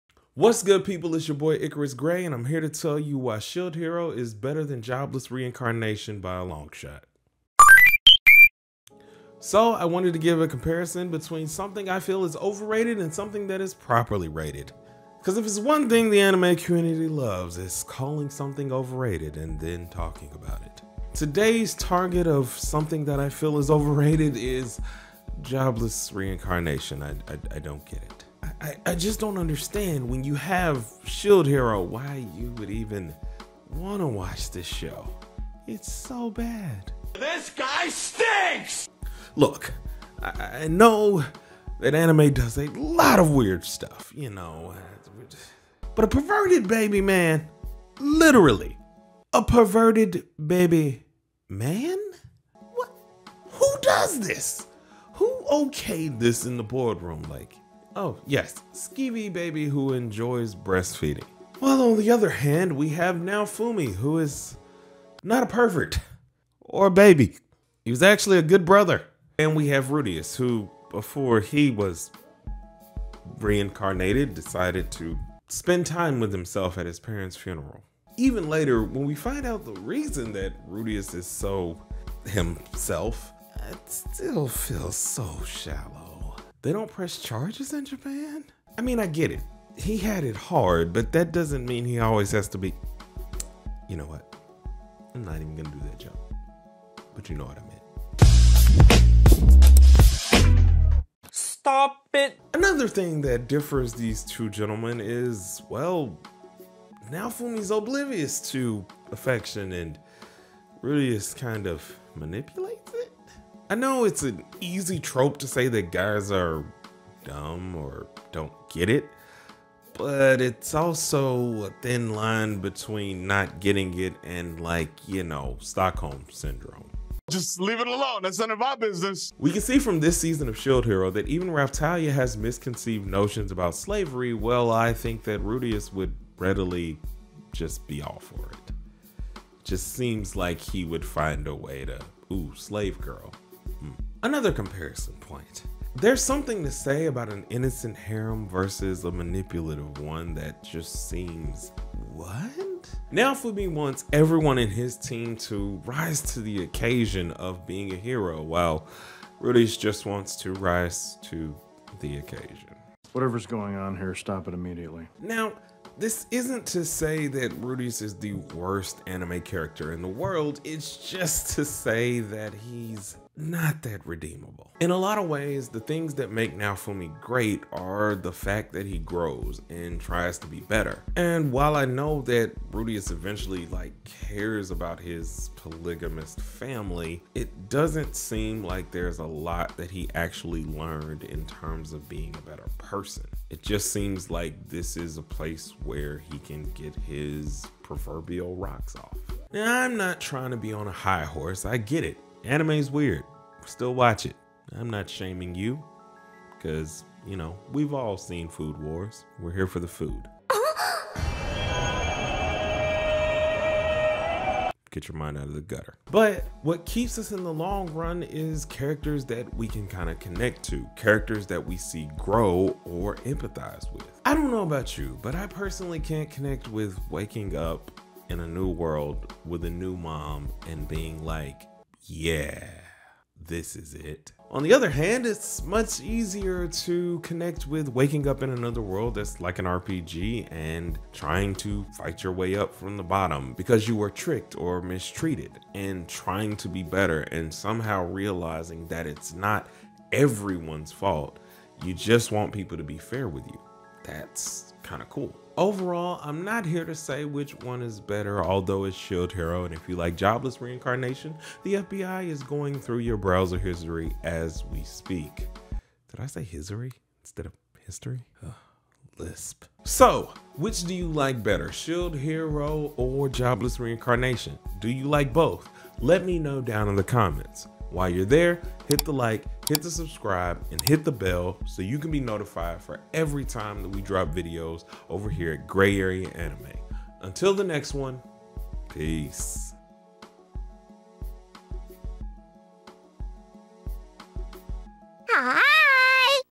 Shield Hero is So Much Better than Jobless Reincarnation
After watching Mushoku Tensei I felt like doing a quick comparison video on which I think is better between it and Shield Hero.

00:00 - Intro
00:24 - Jobless Reincarnation Vs. Shield Hero
01:44 - Oblivious vs. Stockholm Syndrome
02:25 - Innocent Harem vs. Manipulated Harem
03:19 - My Overall Conclusion
04:15 - Outro

All Affliate Links Are Sponsored and Generate Revenue For Gray Area Anime